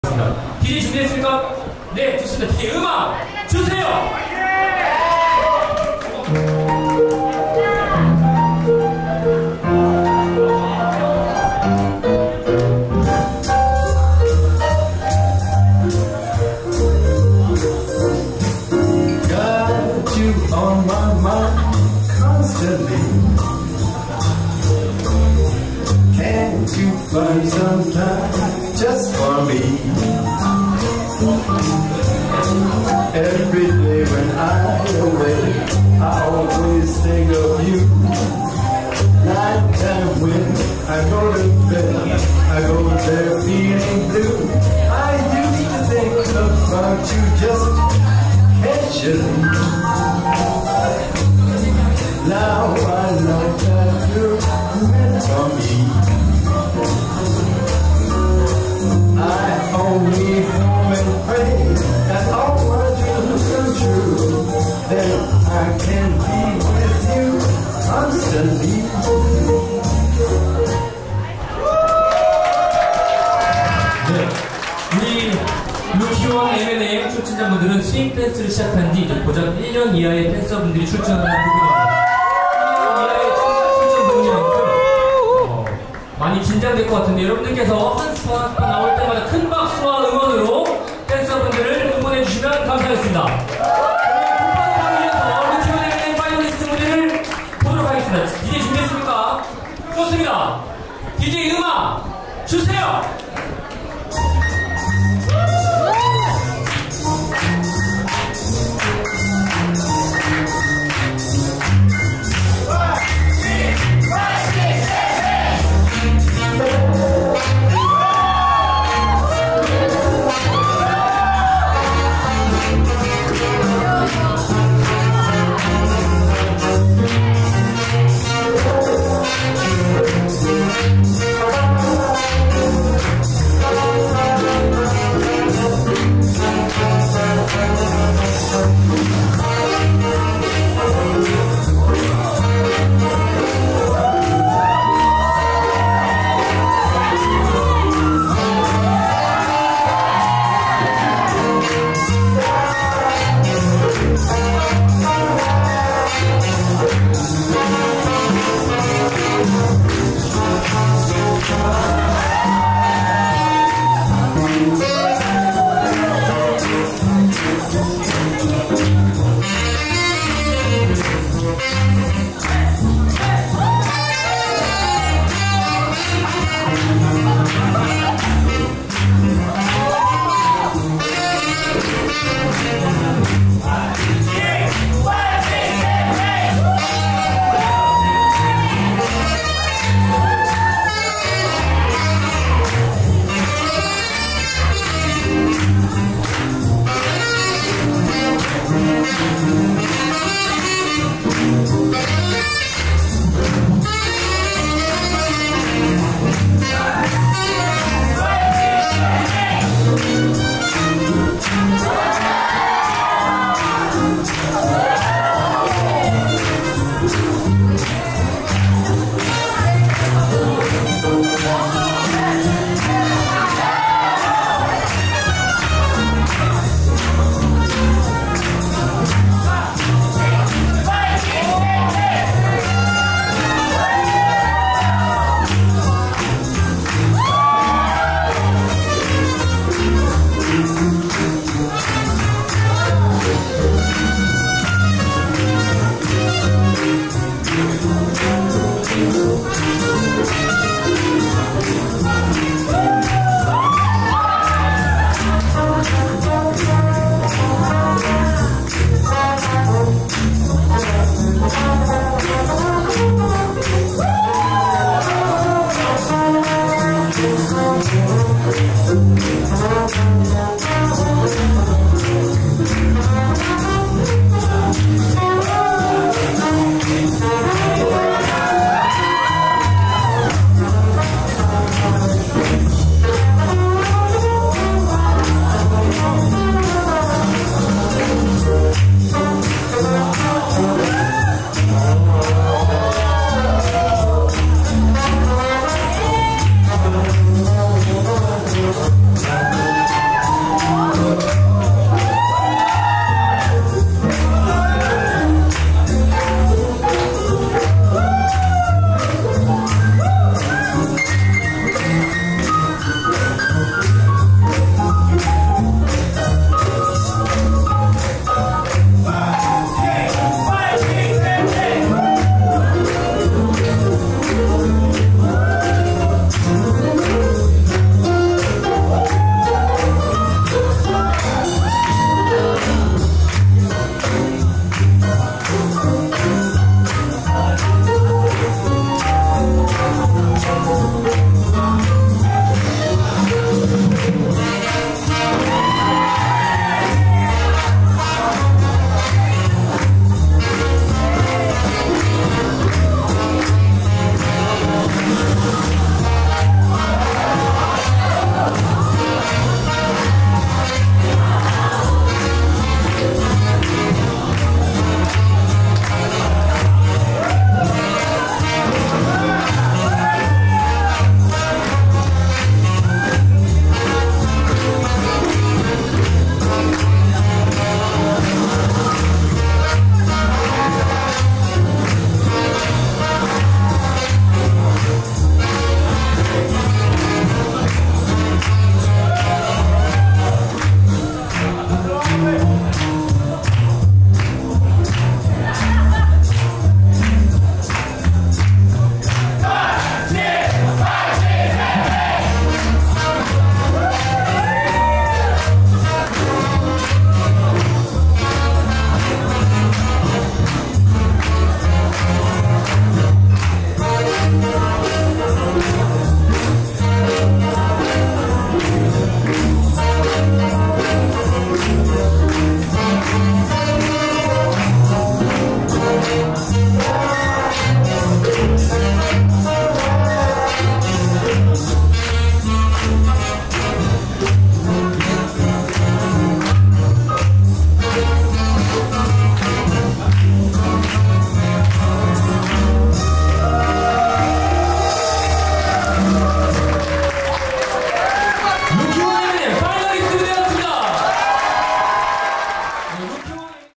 CLEF 2023 - rookie 1 M&M final
0:00 warm up
1:42 spotlight

1.대수&홀리 (3rd)
2.솔솔&미니미니 (1st)

4.감자&라비앙 (2nd)

4.잠온다&리미
5.스맙&유월
6.이로&마님

CLEF(Chungcheong Lindyhop Exchange Festival)는 청주의 파스텔스윙, 대전의 우주스윙, 천안의 스윙하이 세 동호회가 주관하는 충청권의 린디합 행사입니다.

◆ 23년 9월 9일(토) 아지트 - 충북 청주시 흥덕구 사직대로 10, 3F

◆ Instructors & Judges : 히키 / 안슈 / 로빈 / 끌로이 / 상상예찬 / 월향

◆ DJ : 메이저 / 우키

◆ Photograph : 썸비 / 이응

◆ Competition
       Rookie1 M&M
       Rookie2 M&M
       Open M&M
       Advanced M&M
       Open Strictly
       Solo Jazz